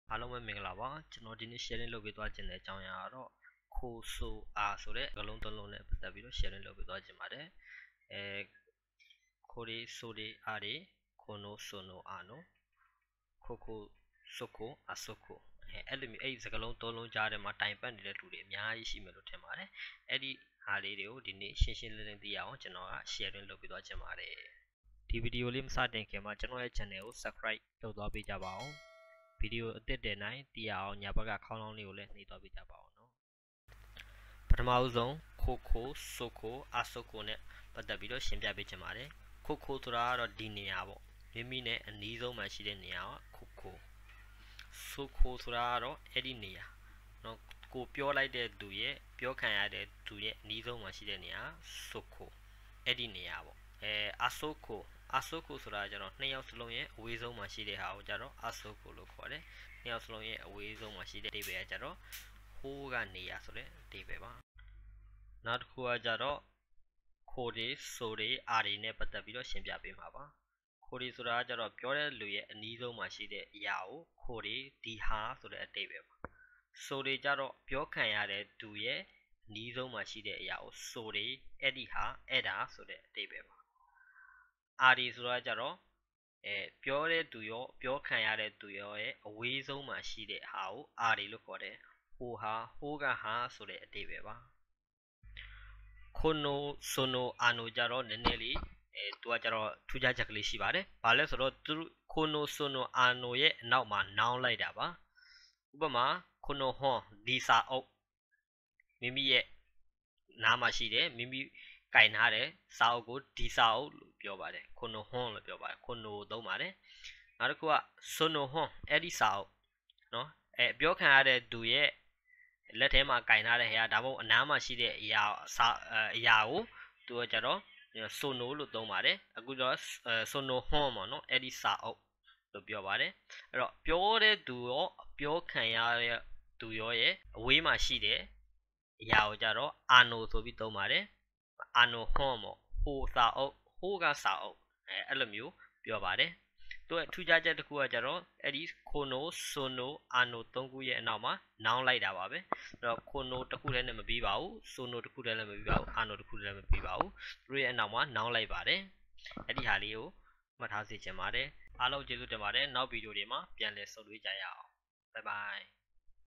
What is Kore Sore Are? How to use? When? ( ko so a)「こ そ あ」Japanese Juku
今日は　こ　そ　あ　について説明したいと思います。
この動画は面白かった、役に立ったと思ったら「いいね！

フェイスブックページにも　いいね！、コメント、シャア　をお願い致します。